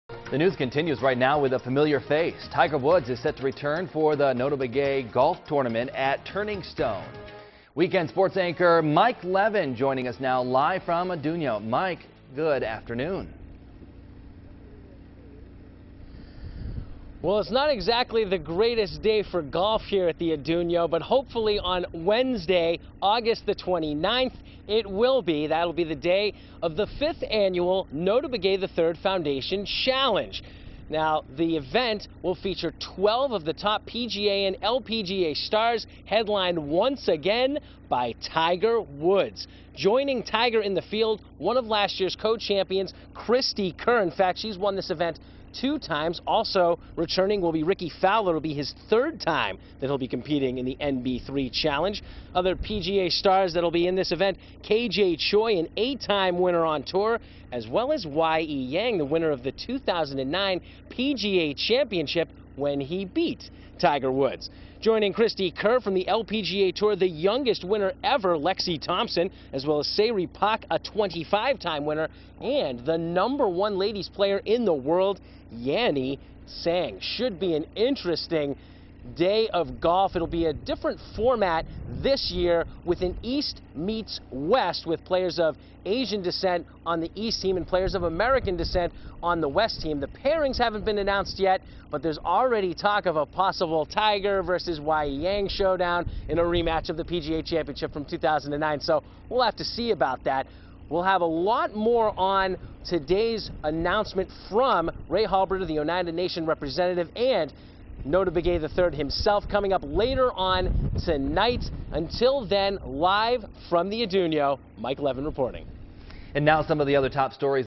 NB3 Announcement Live Shot
WKTV's Mike Levin live at the Atunyote Golf Club after the press conference announcing the date and field for the 2012 NB3 Challenge, including Tiger Woods.